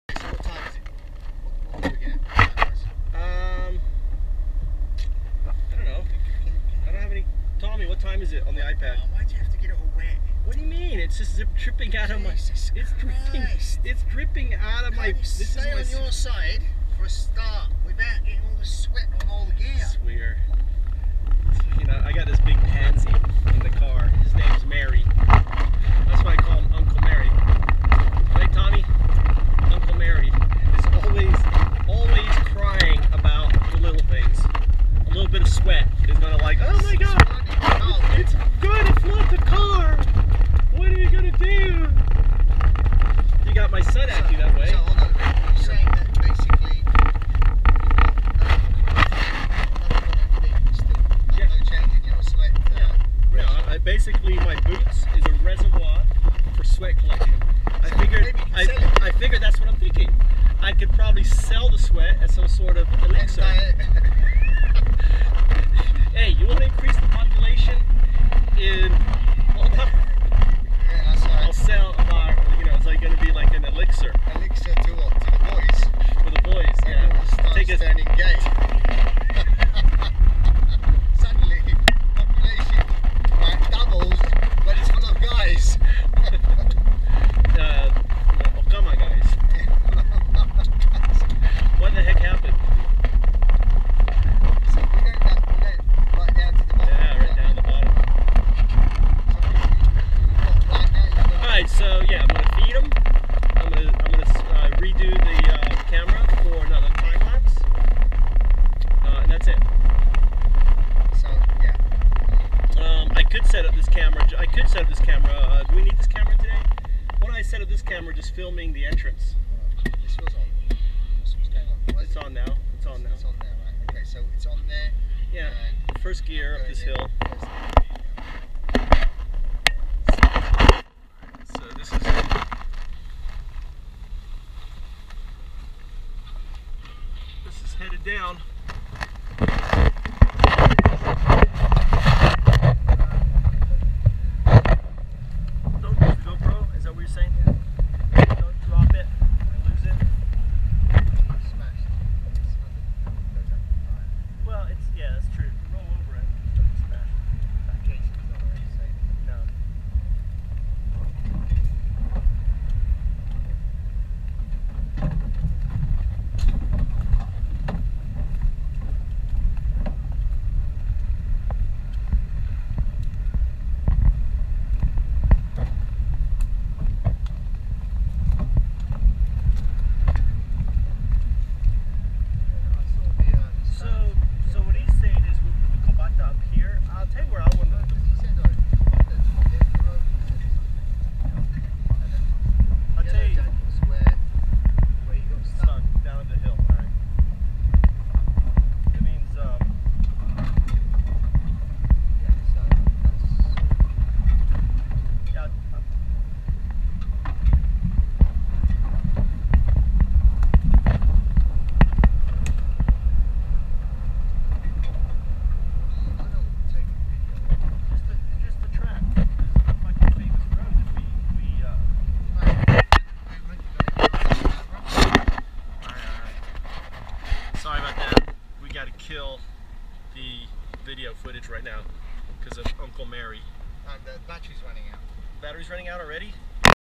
Struggling with uncle Mary on Smithsonian killerhornet shoot.
With my assistant Marius and Tommy in the back. Talking about camera's  Headed down to the filming location. Shikoku Japan.

I have multiple locations with hornets. getting them all ready for the upcoming shoot.